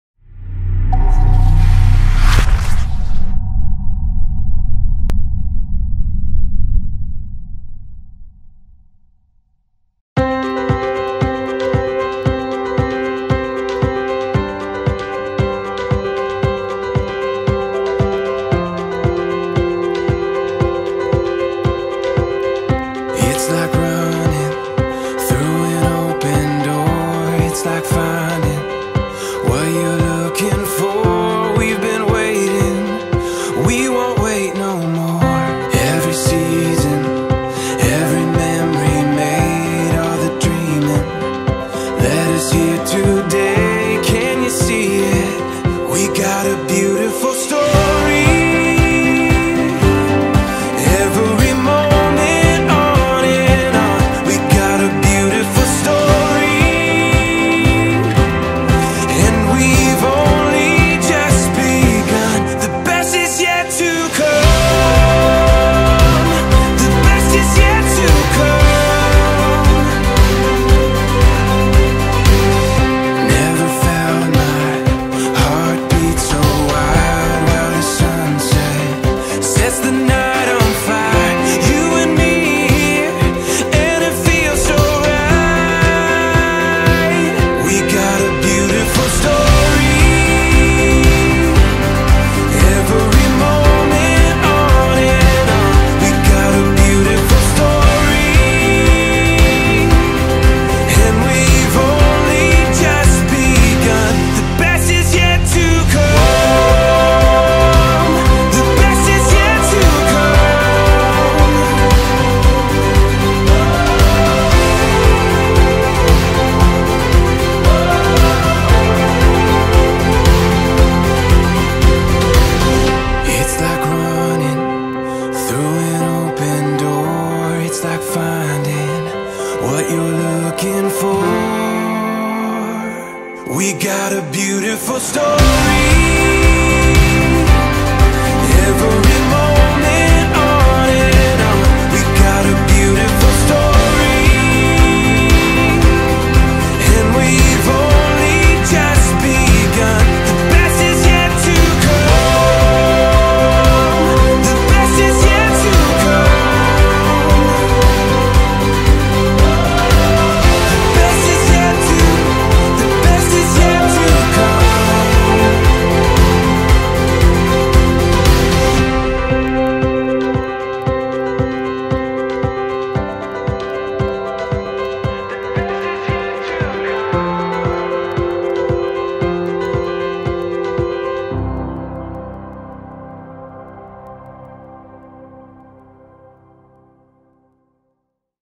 Cinematic Wedding Film in Kolkata | Raktim & Sutanuka | The Wedding Memories
Love does not consist in gazing at each other, but in looking outward together in the same direction...
We loved with a love that was more than love... :)